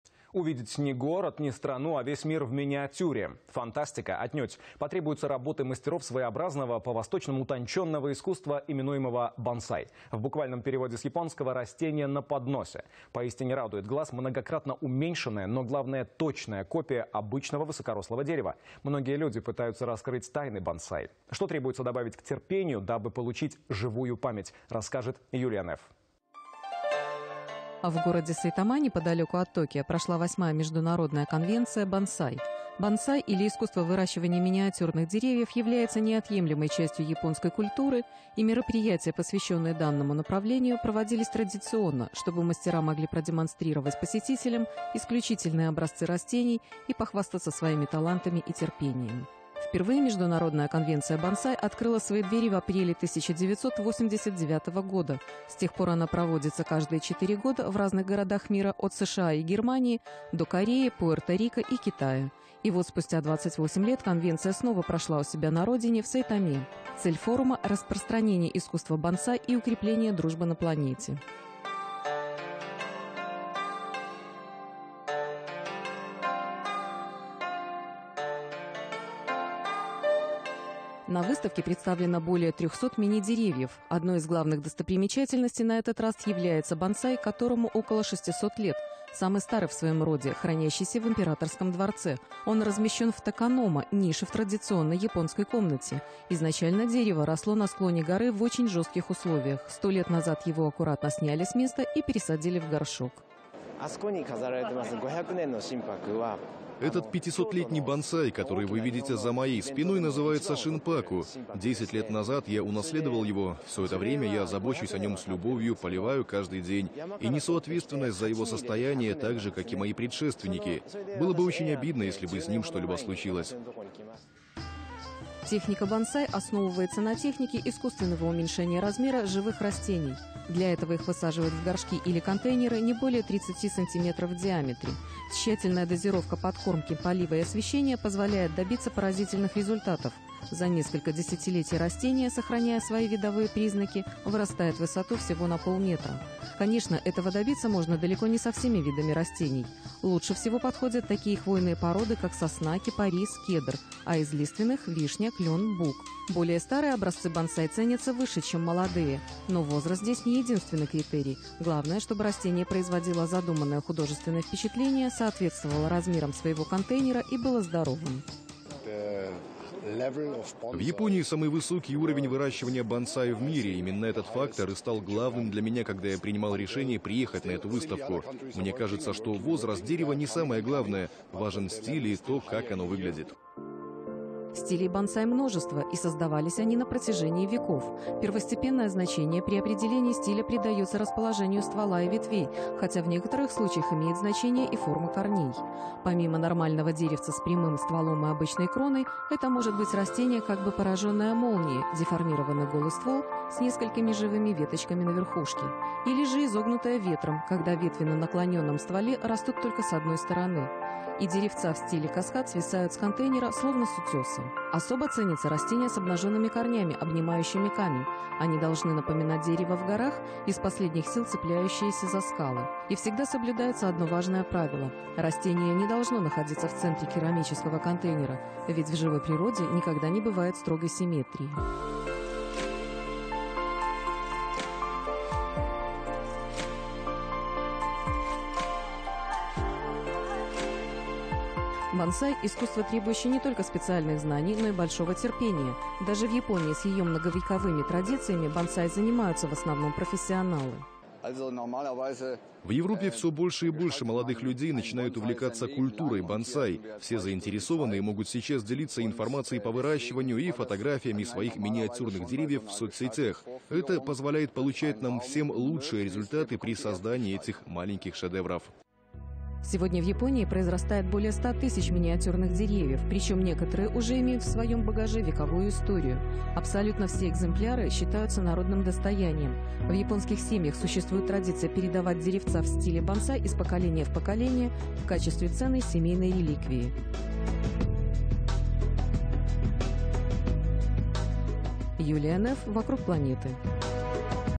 Бонсай: искусство выращивать дерево в миниатюре. Вокруг планеты
Бонсай - часть японской культуры. На недавней выставке мастера смогли похвастаться зрителям своими достижениями. Ведь это кропотливая работа. Для миниатюр подходят не так много деревьев, среди которых сосна, кипарис, кедр, вишня и бук.
Кстати, на выставке был представлен самый старый в своем роде бонсай. Ему около 600 лет. Он хранится в императорском дворце.